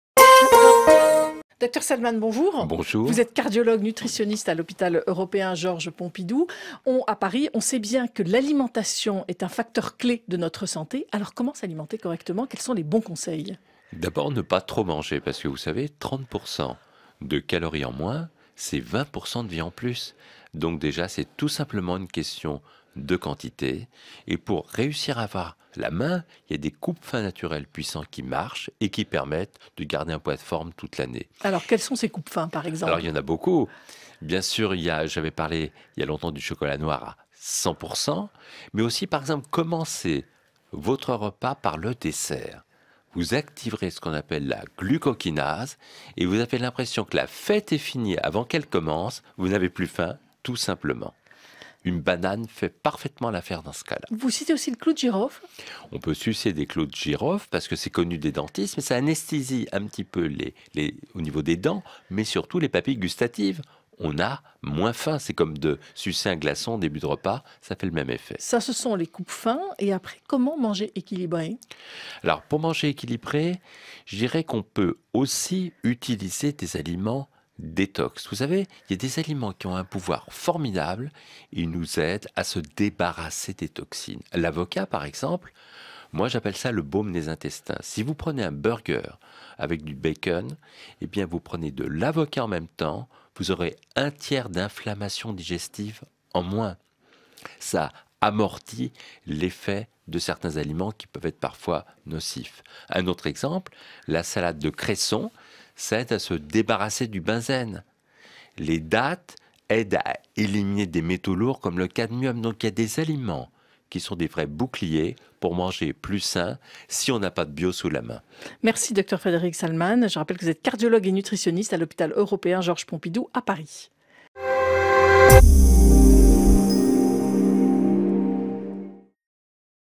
Equilibrer son alimentation
5 fruits et légumes par jour, moins de gras et de sucre, plus de légumineuses… les messages prônant une alimentation équilibrée se multiplient. Comment s’y retrouver ? Comment se nourrir au quotidien pour rester en bonne santé ?Avec :Dr Frédéric Saldmann, cardiologue et nutritionniste, attaché à l’Hôpital Européen Georges Pompidou à Paris. Auteur  de l’ouvrage Votre santé sans risque, aux éditions Albin Michel.